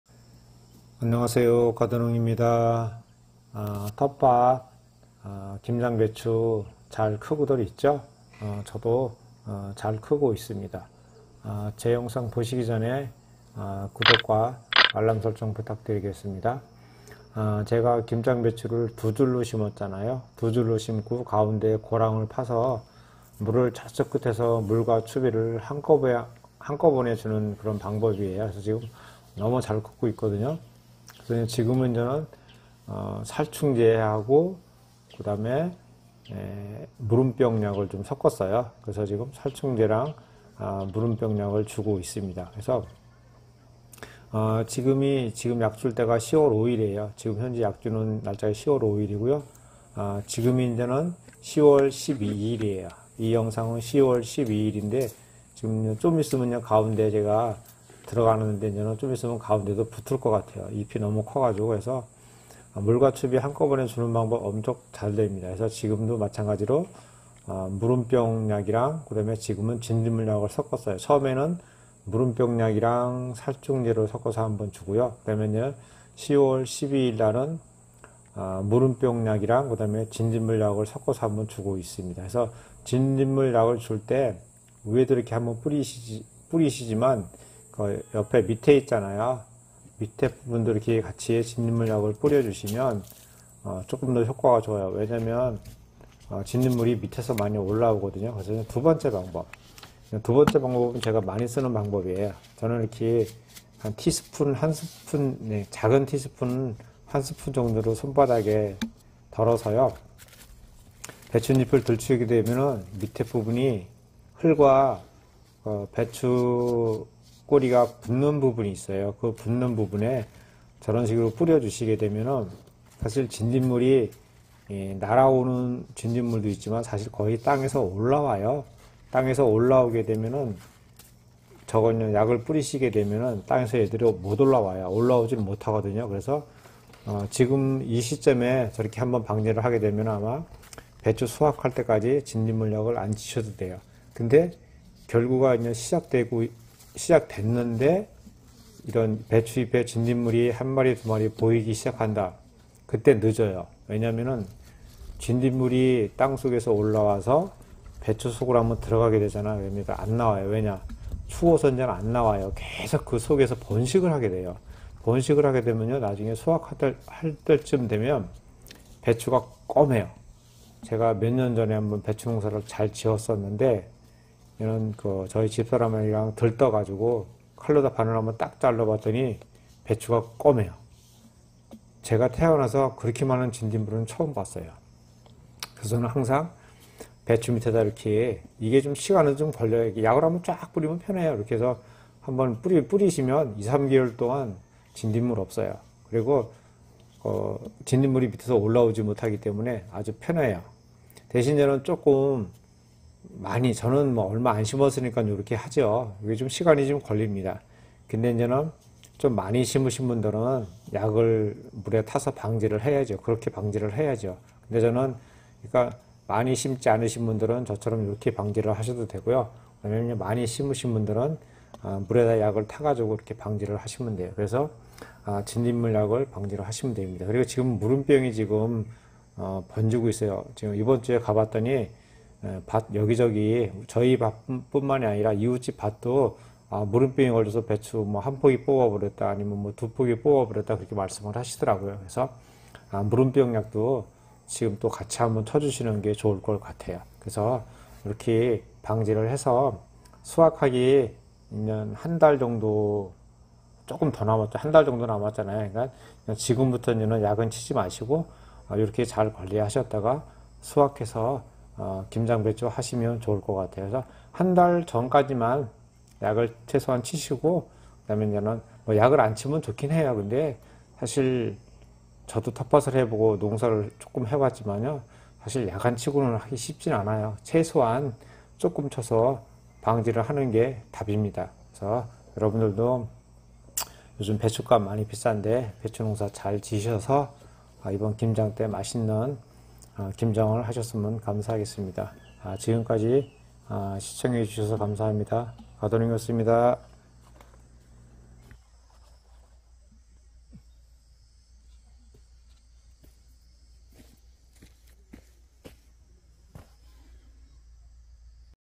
진딧물 과 무름병 지금 꼭 잡으셔야 김장배추 먹을수 있어요,
김장배추는 여러가지 병 해충이 있지만 그중 무름병과 진딧물이 흔하지만 잡지 않으면 먹을수 없는 치명적인 병 해충 같아요,